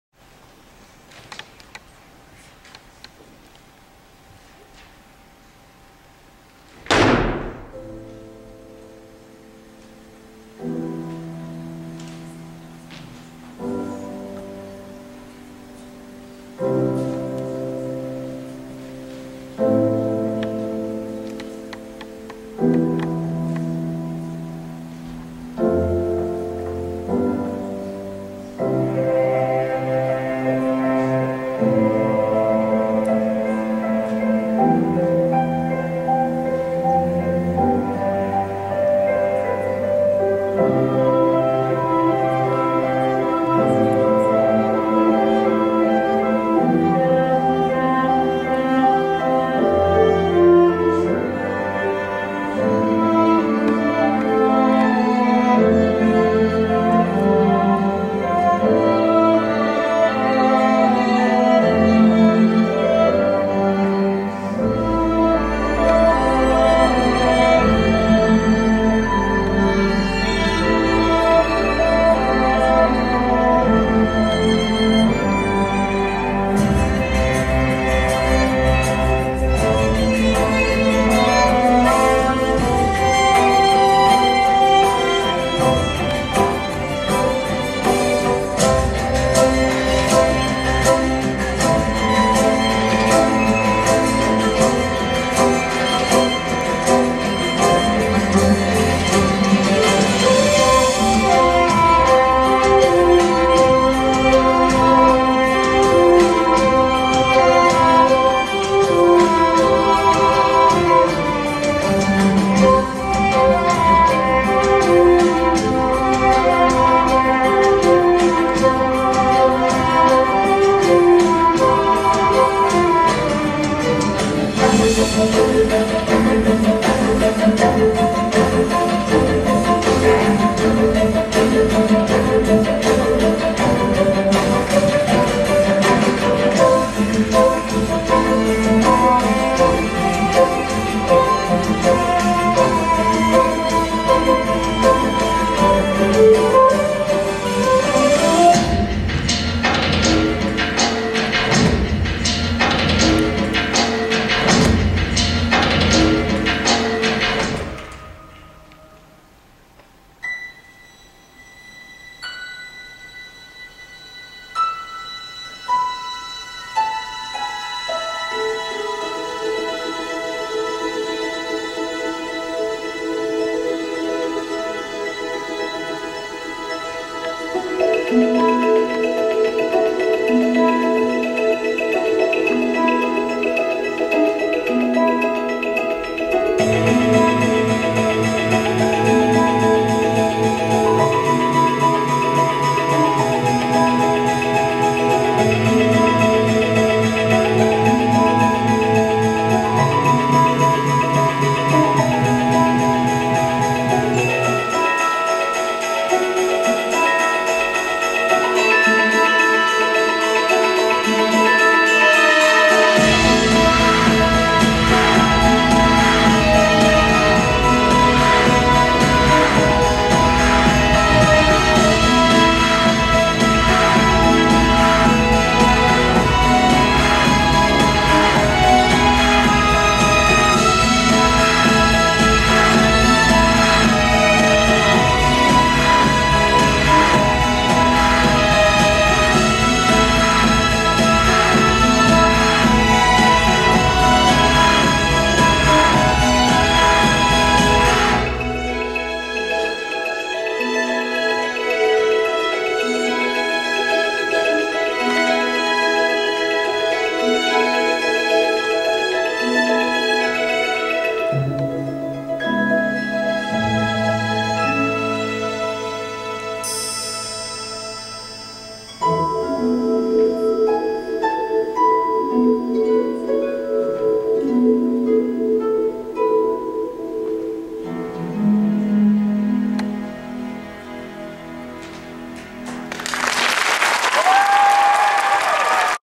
Devon DeSpain Dances at the Chaparral High School Talent Show 4-19-13
Devon DeSpain Dances at the Chaparral High School Talent Show Friday 4-18-13